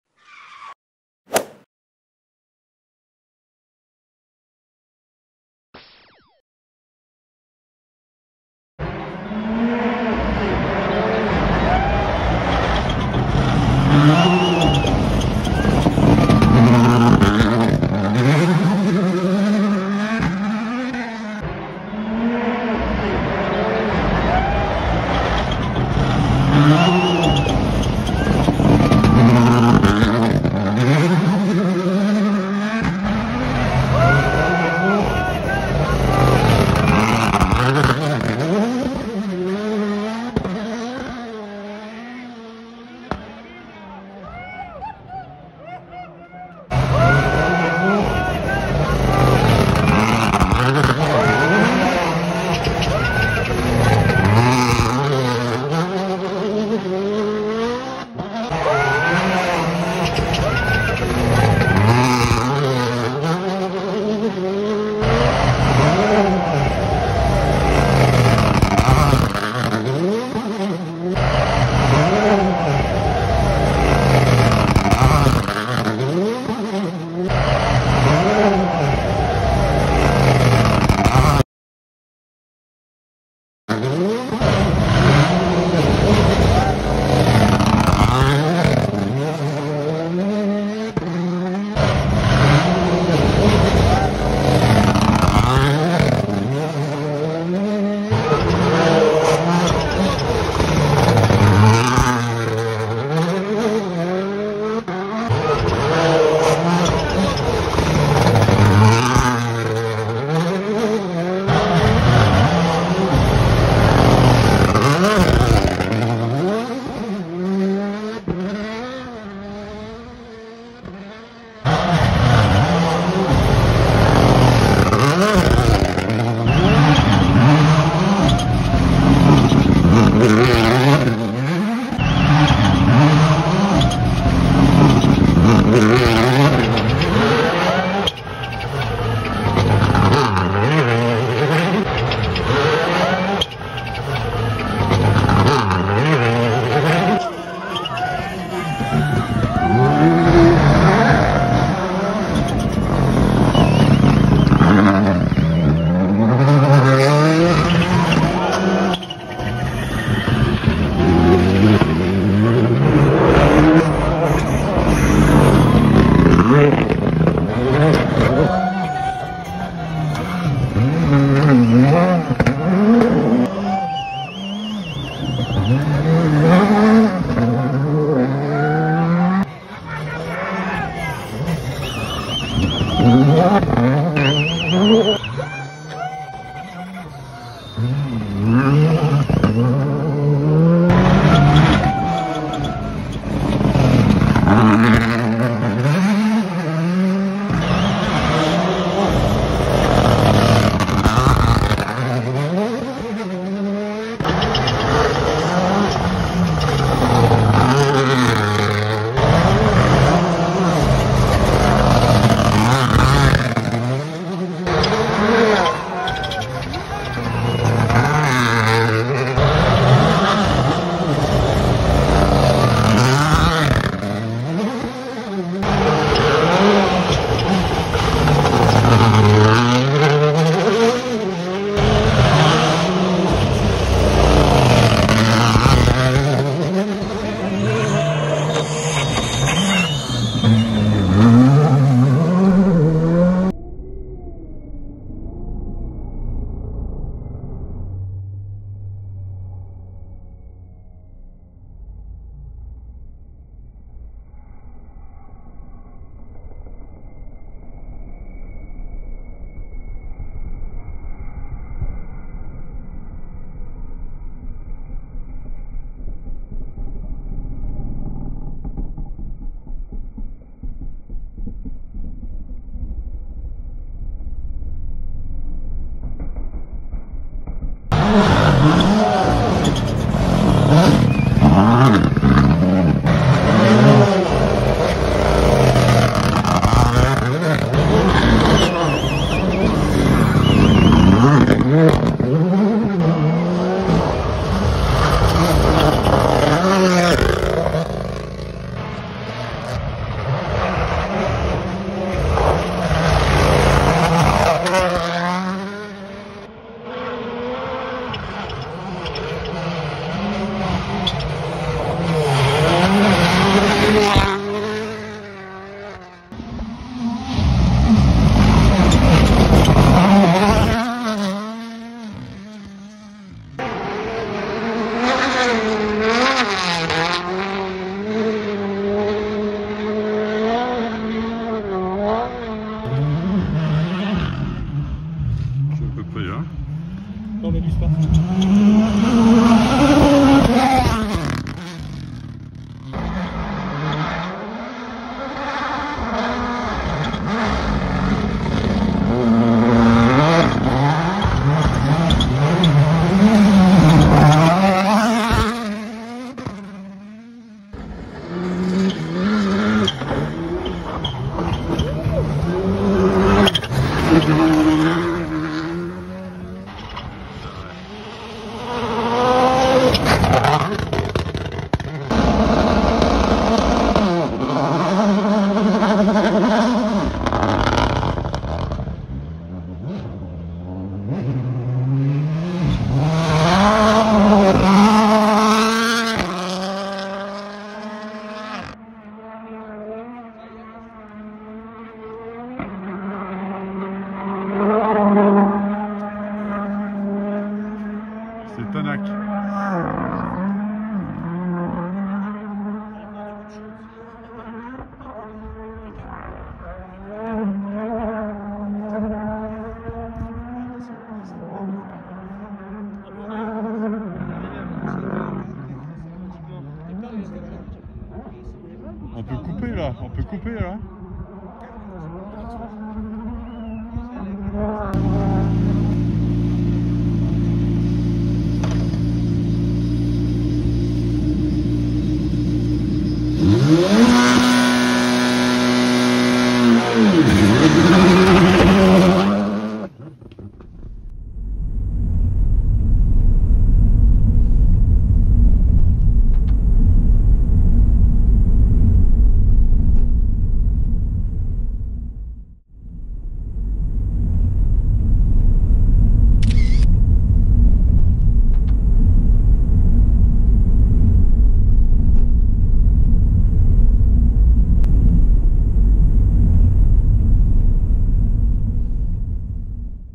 Shakedown, WRC Mexique 2023
La shakedown n’a aucune incidence sur l’épreuve du WRC du Mexique.

SHAKEDOWN. Se dit de la séance d'essais organisée tous les jeudi d'avant rallye, sur un secteur chronométré censé être proche des difficultés au programme du week-end de course. Tous les équipages doivent obligatoirement y passer à trois reprises.

Difficile pour les pilotes de faire des étincelles quand on connaît les limites imposées par la FIA dans la compétition, en pneumatiques, en moteurs …
Le seul intérêt pour les pilotes est de peaufiner les réglages de leur voiture.
Cependant le spectacle doit être au rendez-vous pour les fervents supporters.